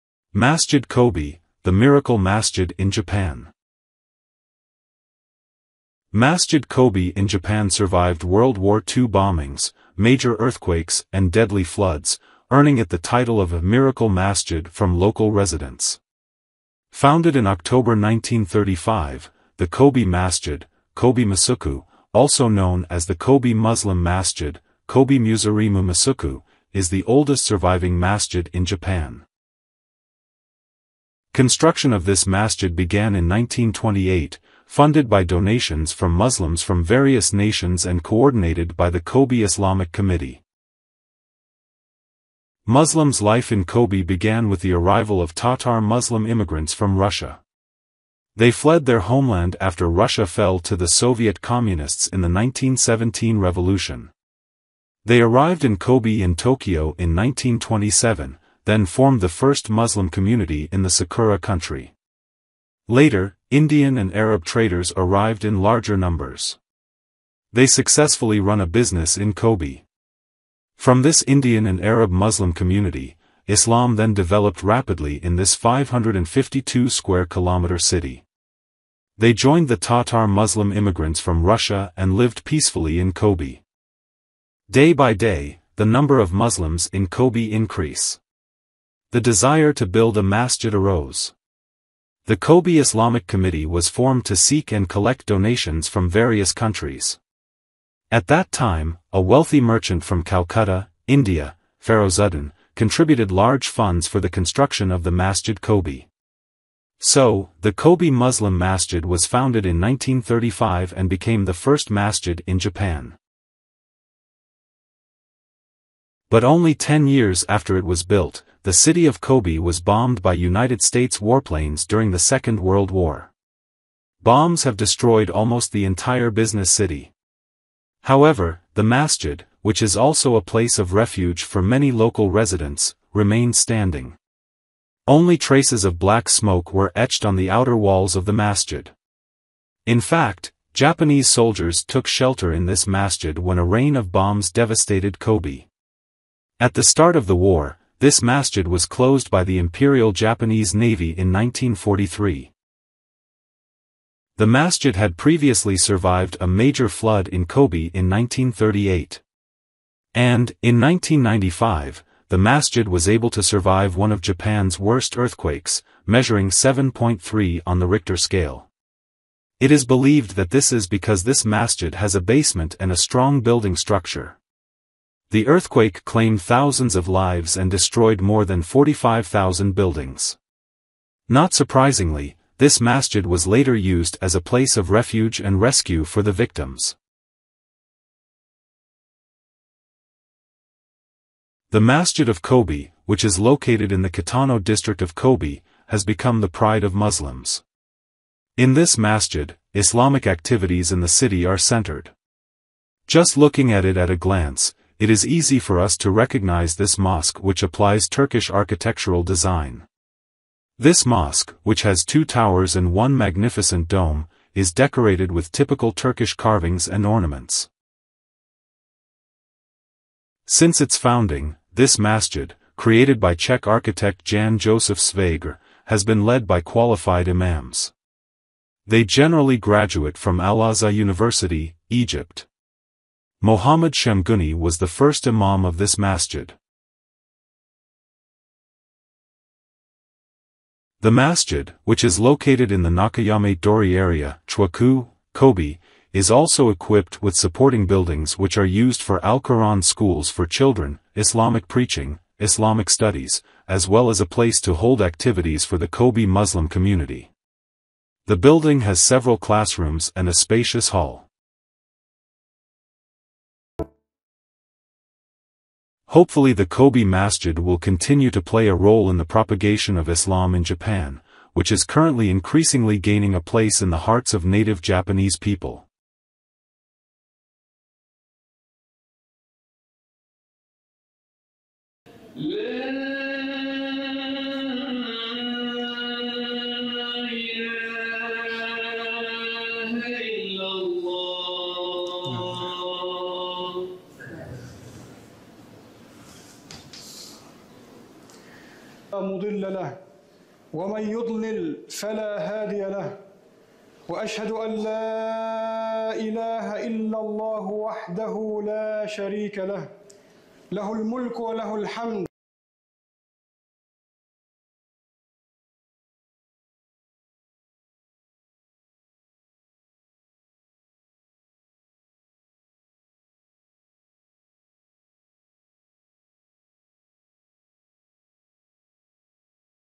Masjid Kobe, The Oldest and Miracle Masjid in Japan | Islam in History
MASJID KOBE, The Oldest and Miracle Masjid in Japan.

Survived in World War II bombings, major earthquakes and deadly floods.